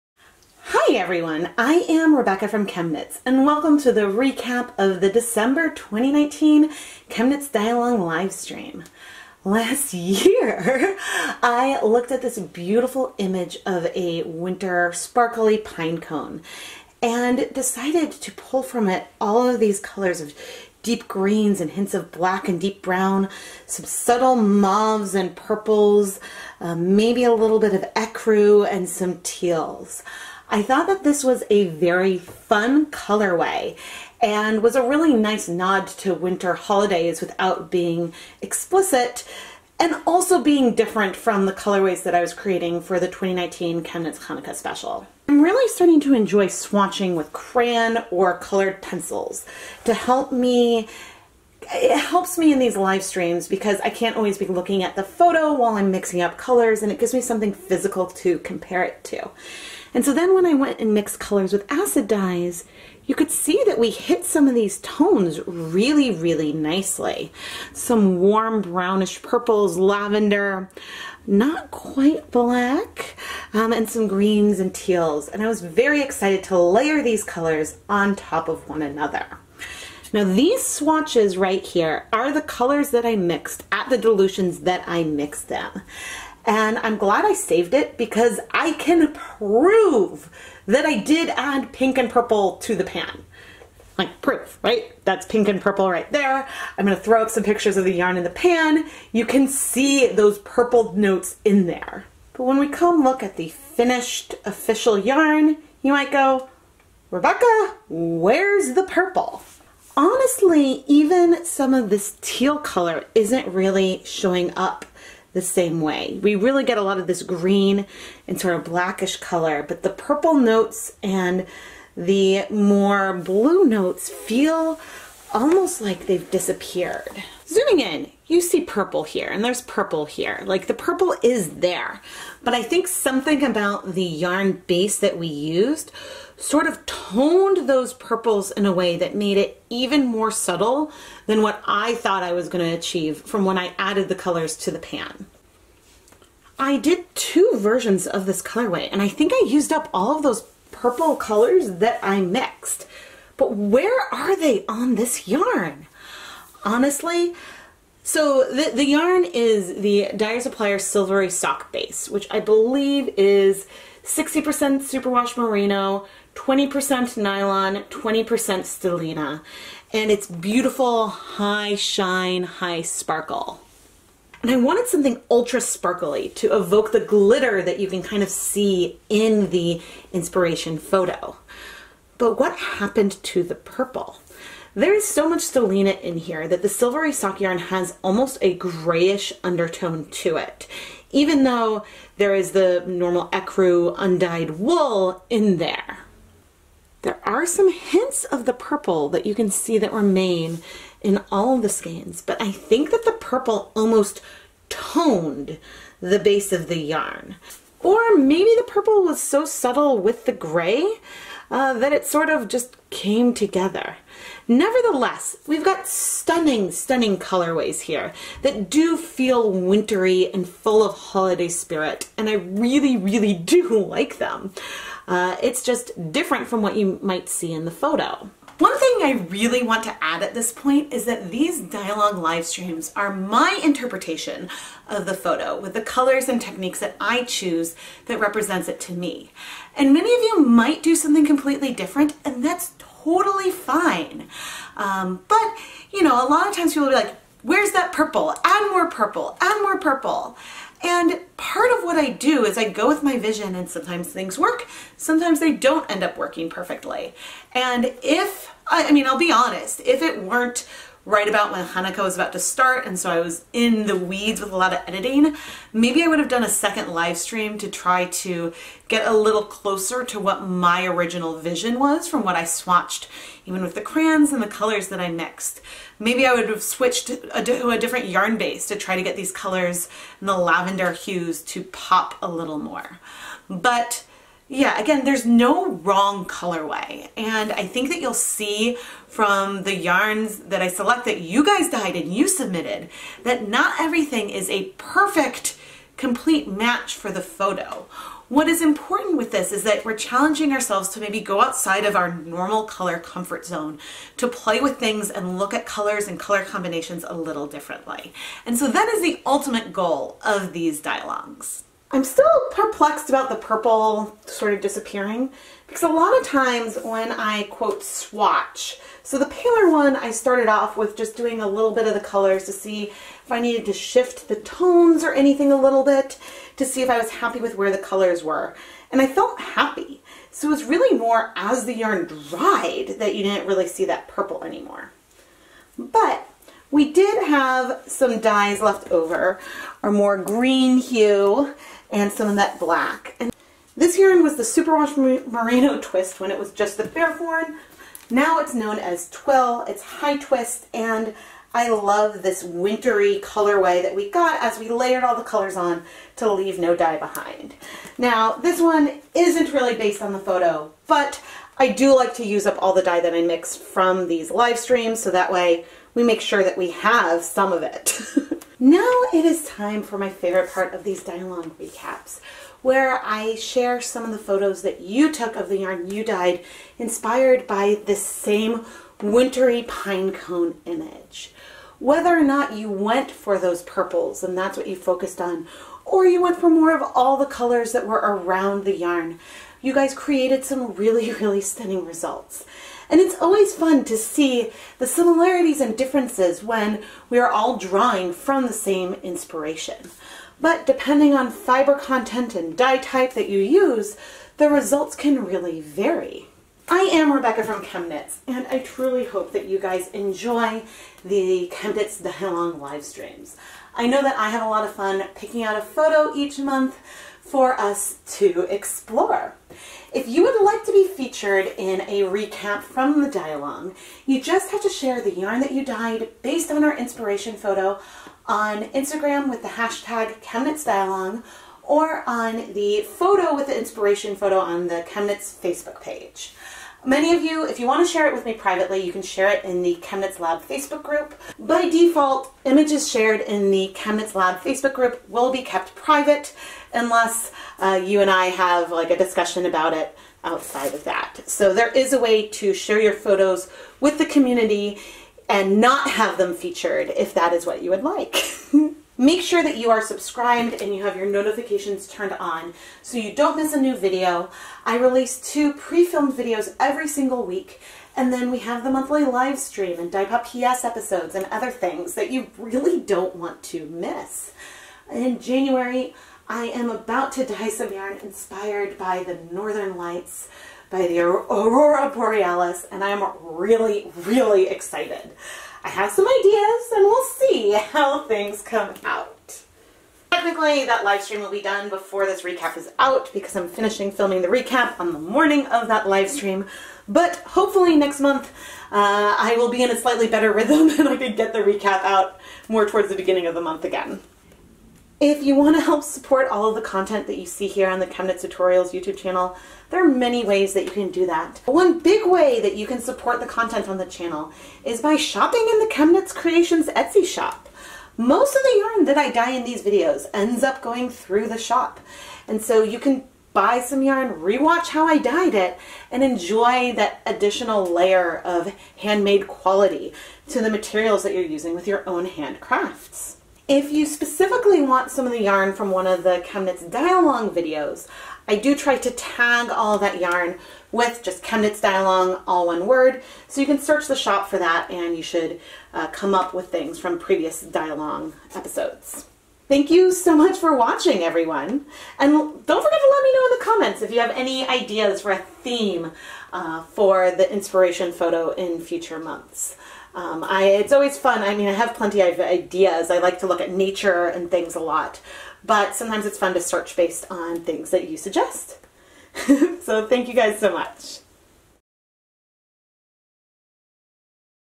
December 2019 ChemKnitsDyeAlong Livestream Recap
Yarns used: Bare Twill Merino Twist (KnitPicks) and Silver Sock yarn (Dyer Supplier)
Dyer Supplier sent me the bare yarn for free to review and test out.
Catering Steam Pan
Purple Nitrile Gloves
Reusable Zip Ties

To be featured in the livestream recap with your own dye along photos, use ChemKnitsDyeAlong on Instagram or reply to the picture on the ChemKnits Facebook